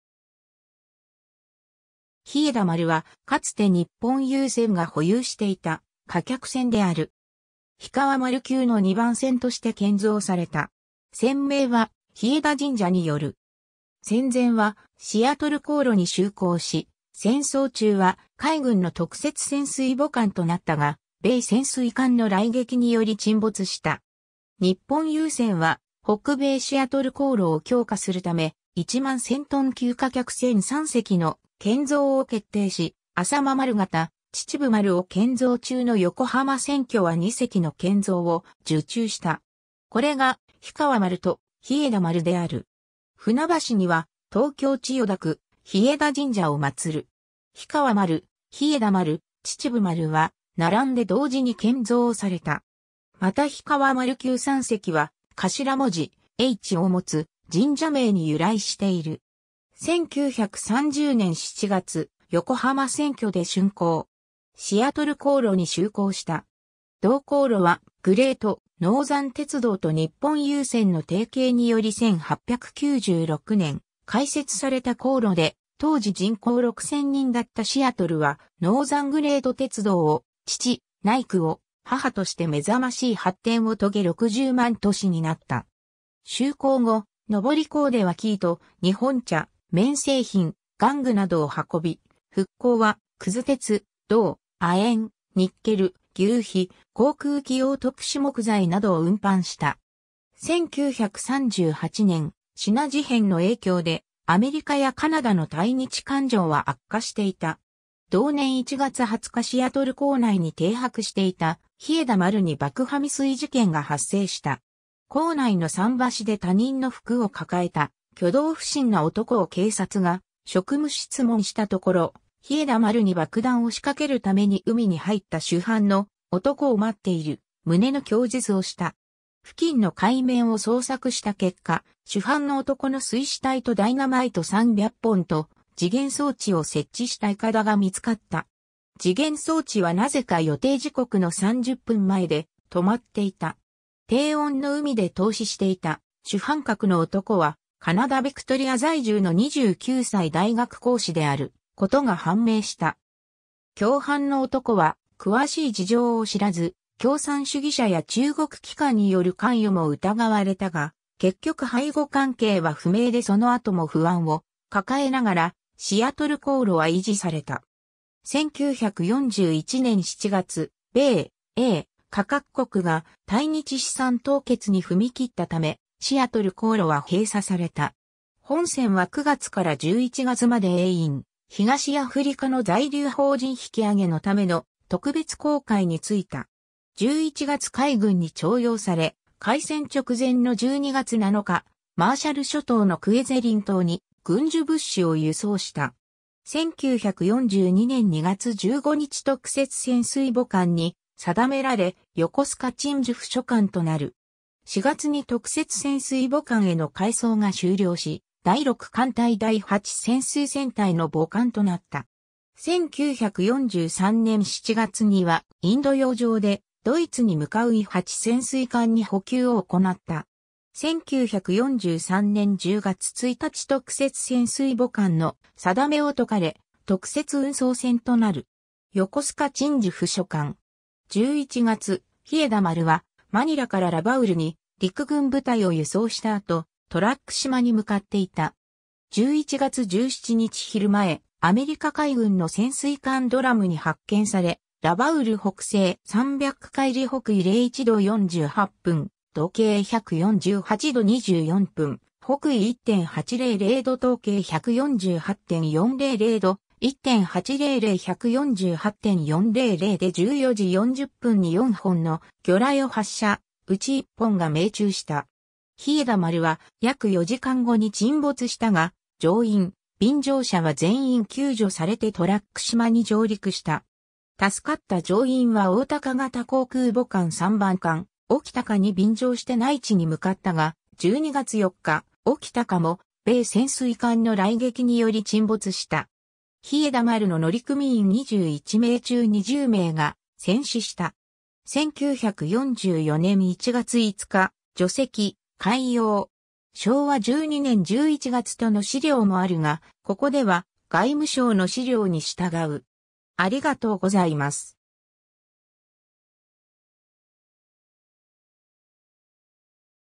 日枝丸
日枝丸（ひえまる）は、かつて日本郵船が保有していた貨客船である。
氷川丸級の2番船として建造された。
船名は日枝神社（東京都千代田区）に依る。
 戦前はシアトル航路に就航し、戦争中は海軍の特設潜水母艦となったが、米潜水艦の雷撃により沈没した。
 日本郵船は北米シアトル航路を強化するため、1万1000トン級貨客船3隻の建造を決定し、浅間丸型「秩父丸」を建造中の横浜船渠は2隻の建造を受注した。
これが「氷川丸」と「日枝丸」（本船）である。
船橋には東京千代田区日枝神社を祀る。
「氷川丸」、「日枝丸」、「秩父丸」は並んで同時に建造された。
また氷川丸級3隻（氷川丸、日枝丸、平安丸）は頭文字『Ｈ』を持つ神社名（氷川神社、日枝神社、平安神宮）に由来している。
 1930年（昭和5年）7月、横浜船渠で竣工。
シアトル航路に就航した。
同航路はグレート・ノーザン鉄道と日本郵船の提携により1896年（明治29年）開設された航路で、当時人口6000人だったシアトルは「ノーザングレート鉄道を父、NYK（日本郵船）を母」として目覚ましい発展を遂げ60万都市になった。
 就航後、往行では生糸、日本茶、綿製品、玩具などを運び，復航は屑鉄、銅、亜鉛，ニッケル、牛皮、航空機用特殊木材などを運搬した。
 1938年（昭和13年）、支那事変の影響でアメリカやカナダの対日感情は悪化していた。
同年1月20日シアトル港内に停泊していた日枝丸に爆破未遂事件が発生した。
 港内の桟橋で他人の服を抱えた挙動不審な男を警察が職務質問したところ、日枝丸に爆弾を仕掛けるために海に入った主犯の男を待っている旨の供述をした。
付近の海面を捜索した結果、主犯の男の水死体とダイナマイト300本と時限装置を設置した筏が見つかった。
時限装置はなぜか予定時刻の30分前で止まっていた。
低温の海で凍死していた主犯格の男は、カナダ ビクトリア在住の29歳大学講師であることが判明した。
共犯の男は詳しい事情を知らず、共産主義者や中国機関による関与も疑われたが、結局背後関係は不明でその後も不安を抱えながらシアトル航路は維持された。
 1941年（昭和16年）7月、米・英・加 各国が対日資産凍結に踏み切ったためシアトル航路は閉鎖された。
本船は9月から11月まで英印・東アフリカの在留邦人引き揚げのための特別航海についた。
11月海軍に徴用され、開戦直前の12月7日、マーシャル諸島のクェゼリン島に軍需物資を輸送した。
 1942年（昭和17年）2月15日特設潜水母艦に定められ横須賀鎮守府所管となる。
4月に特設潜水母艦への改装が終了し、第6艦隊第8潜水戦隊の母艦となった。
1943年（昭和18年）7月にはインド洋上で、ドイツに向かう伊8潜水艦に補給を行なった。
 1943年（昭和18年）10月1日特設潜水母艦の定めを解かれ、特設運送船（雑用船）となる。
横須賀鎮守府所管。
 11月、日枝丸はマニラからラバウルに陸軍部隊を輸送した後、トラック島に向かっていた。
11月17日昼前、アメリカ海軍の潜水艦ドラムに発見され、ラバウル北西300海里 北緯01度48分 東経148度24分 / 北緯1.800度 東経148.400度 / 1.800; 148.400で14時40分に4本の魚雷を発射、内1本が命中した。
日枝丸は約4時間後に沈没したが、乗員・便乗者は全員救助されてトラック島に上陸した。
助かった乗員は大鷹型航空母艦3番艦「冲鷹」（旧、新田丸）に便乗して内地に向かったが、12月4日「冲鷹」も米潜水艦の雷撃により沈没した。
日枝丸の乗組員21名中20名が戦死した。
 1944年（昭和19年）1月5日、除籍・解傭。
 ^ 昭和12年11月との資料もあるが、ここでは外務省の資料に従う